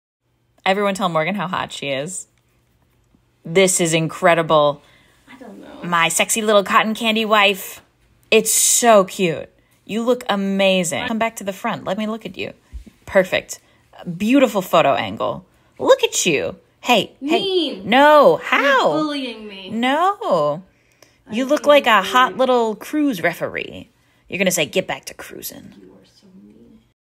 Cruise Fits Sneak Peak
We found the cutest bathing suit for Morgan at Goodwill, it's originally from Target!